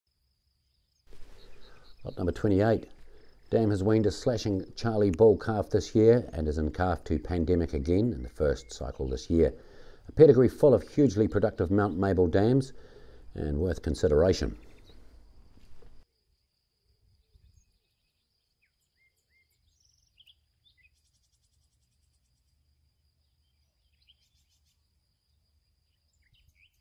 Lot 28 Mt Mable 2212 PV
Dam has weaned a slashing Charlie bull calf this year and is in calf to Pandemic again in the first cycle this year. A pedigree full of hugely productive Mt Mable dams. Worth consideration.